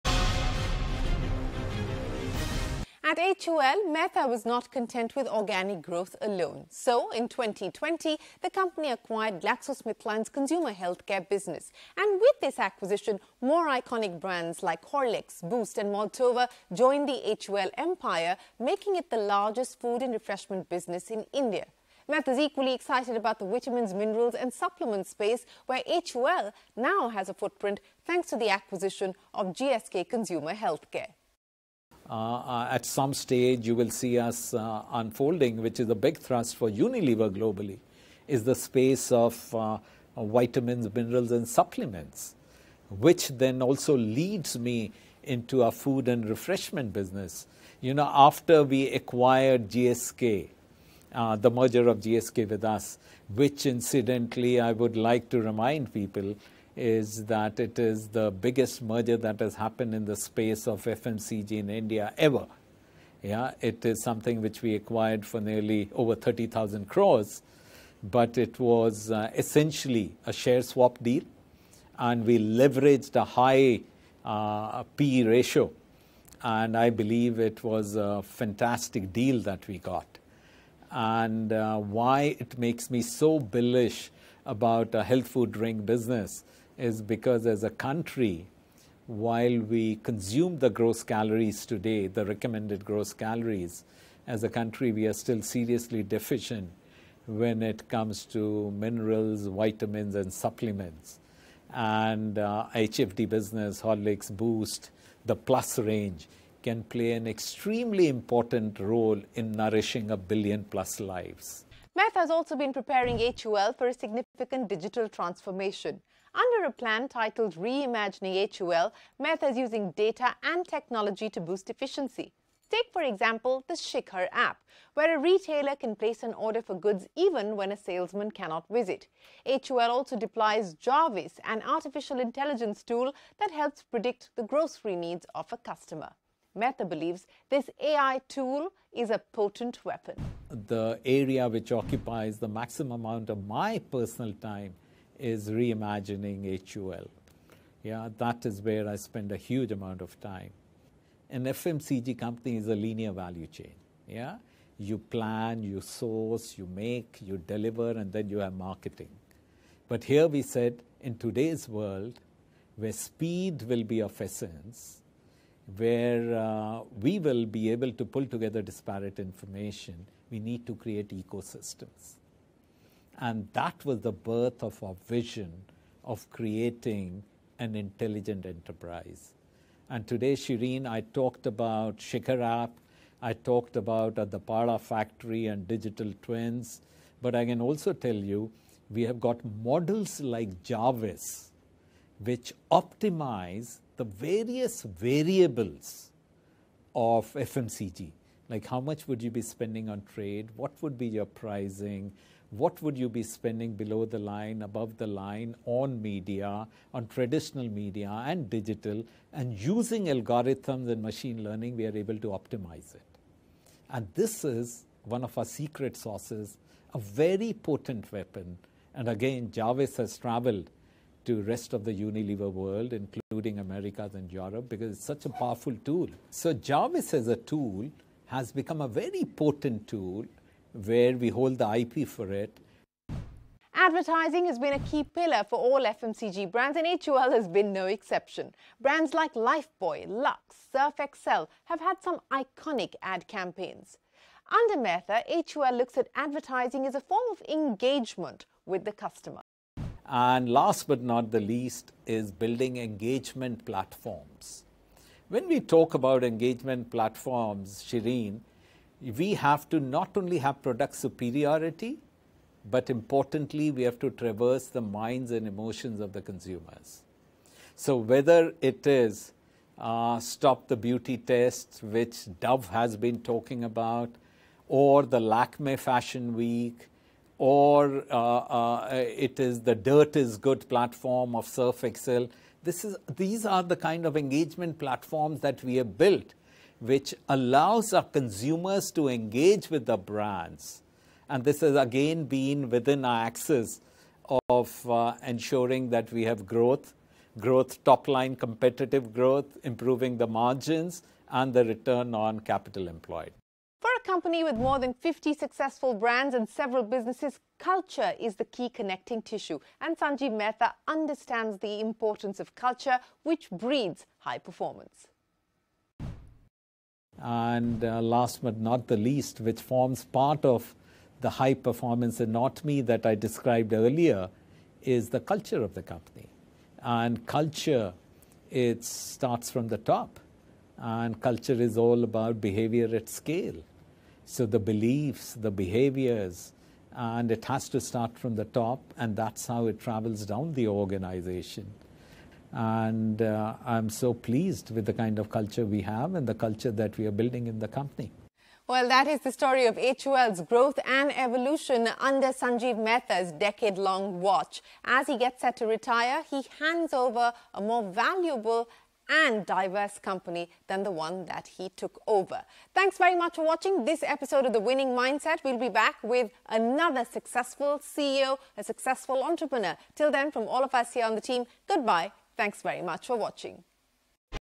HUL's Sanjiv Mehta Talks About Embracing Digital Transformation | Winning Mindset- The CEO Guidebook
Sanjiv Mehta, the outgoing chairman, of Hindustan Unilever (HUL) talks about creating engagement via advertising and more. Listen in.

n18oc_business